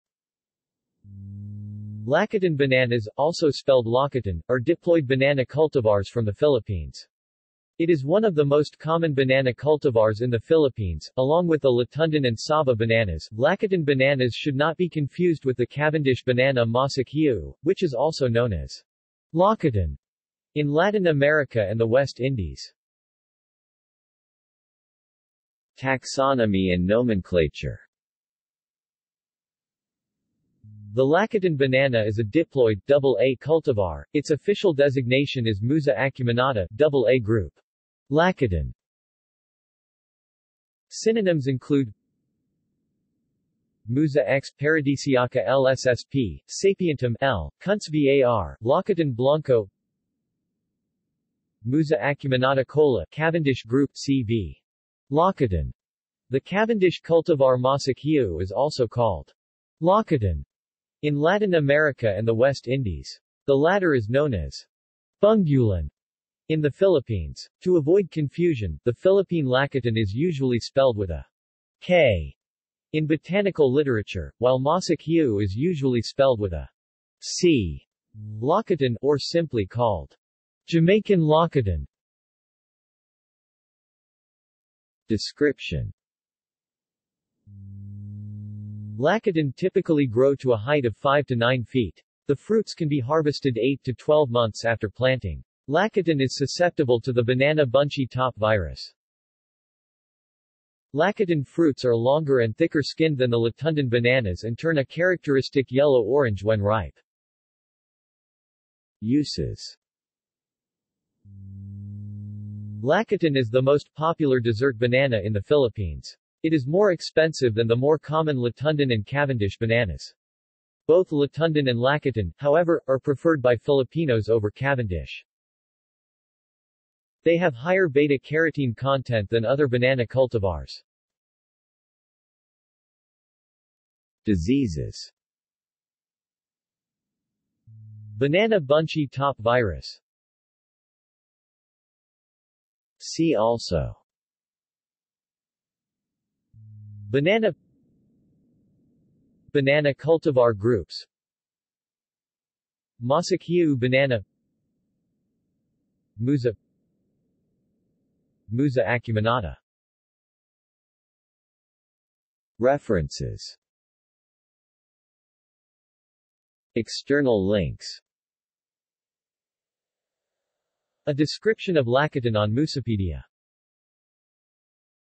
Lacatan banana | Wikipedia audio article
This is an audio version of the Wikipedia Article:
Lacatan banana

SUMMARY
Lakatan bananas, also spelled Lacatan, are diploid banana cultivars from the Philippines. It is one of the most common banana cultivars in the Philippines, along with the Latundan and Saba bananas.Lakatan bananas should not be confused with the Cavendish banana Masak Hijau, which is also known as "Lacatan" in Latin America and the West Indies.